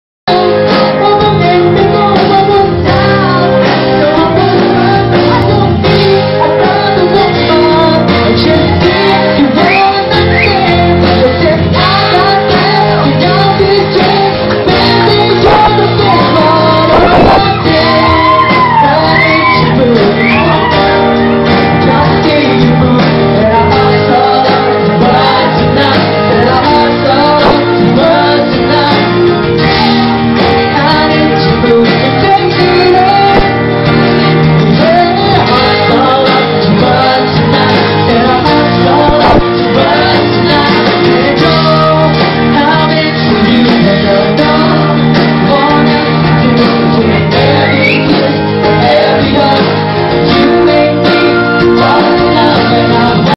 Joma singing with This Side Up Band at Subarus 2
continuation of Joma singing with This Side Up. a little alcohol goes a long way. hehehe. joke.